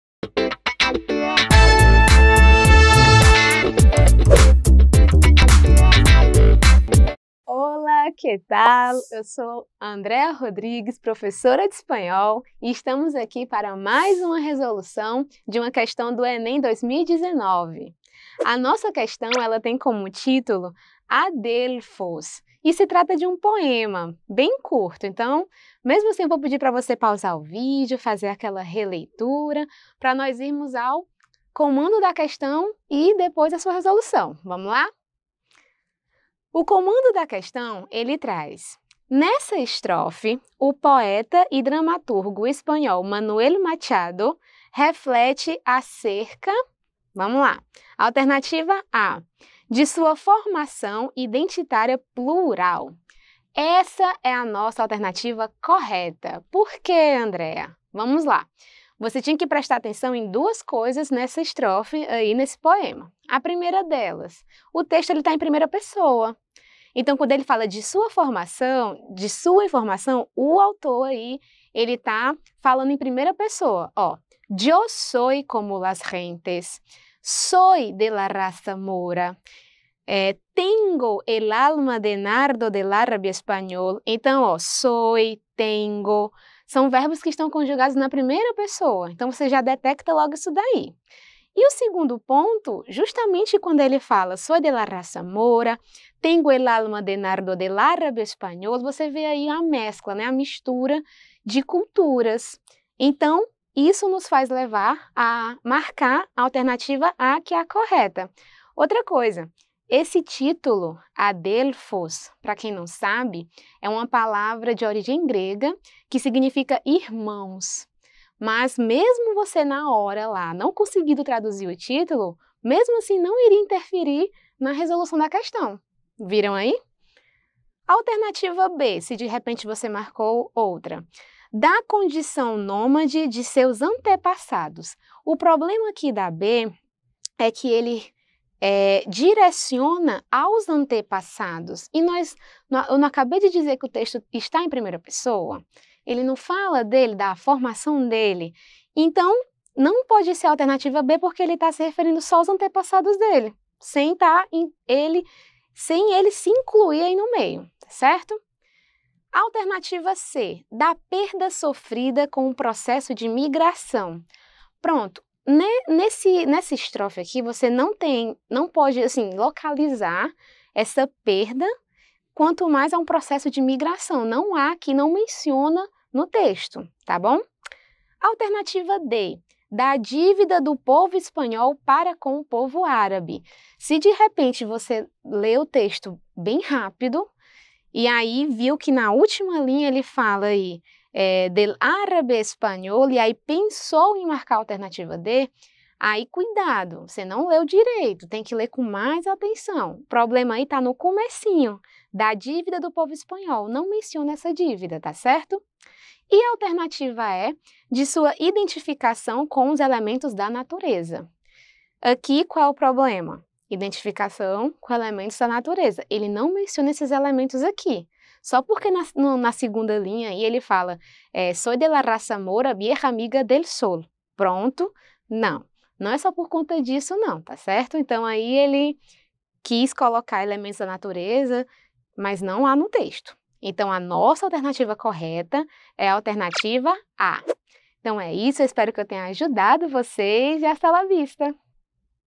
ENEM 2019 | Questão 1 | LC - Língua Espanhola | Prova Azul
Nessa estrofe, o poeta e dramaturgo espanhol Manuel Machado reflete acerca...
Prof. Andrea Rodrigues

GABARITO A

EQUIVALÊNCIA: PROVA AZUL 1  /  PROVA AMARELA 3  /  PROVA ROSA 5  /  PROVA BRANCA 2  /  PROVA CINZA